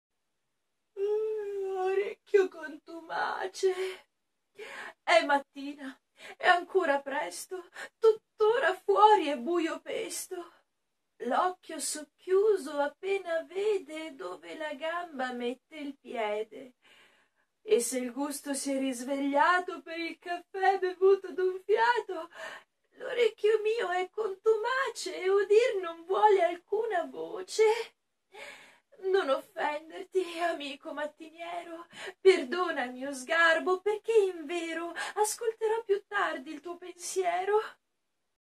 Orecchio contumace
Divertissement delle sorelle Garbo.

Noemi Garbo, Ladypiperita, interpreta i componimenti di Sara Garbo, La Poetessa Della Bassa.

Che fatica il risveglio...vi sveliamo un segreto: se vedete le sorelle Garbo arzille e chiacchierone di prima mattina, vuol dire che stanno facendo un enorme esercizio di lavoro su loro stesse...e che vi vogliono molto bene! 😴😂